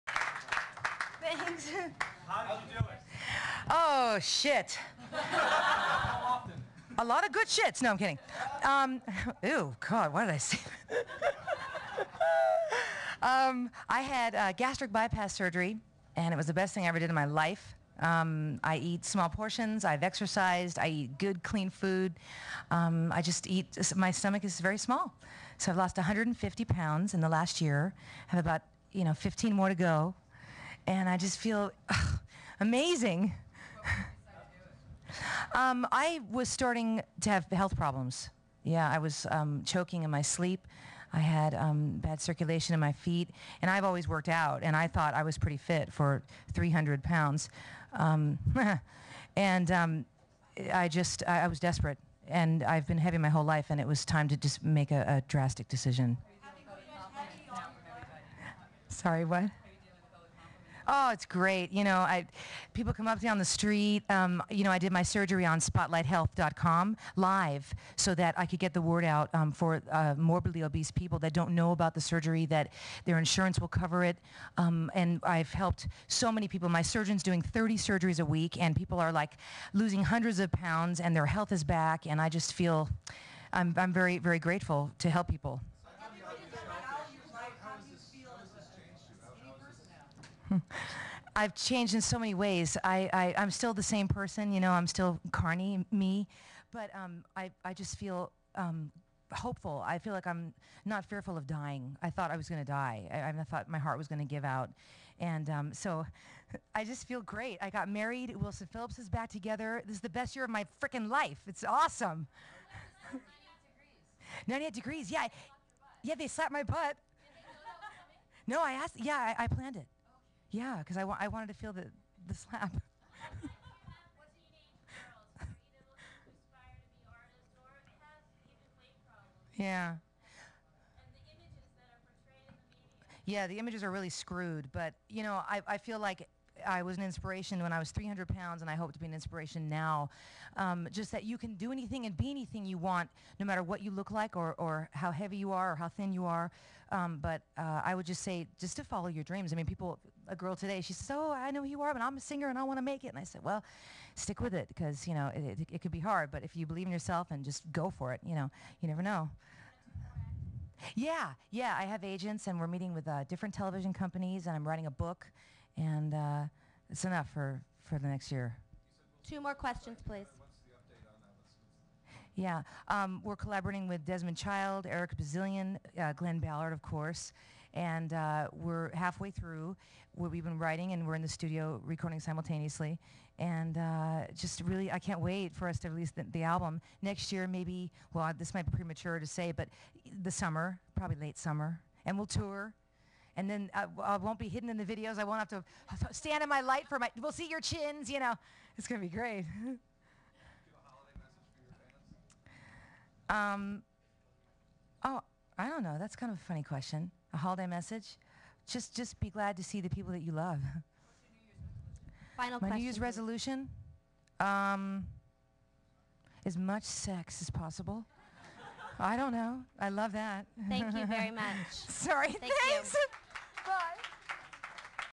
Carnie Wilson Interview (Dec/2000)
Carnie backstage @ the 2000 Billboard Music Awards.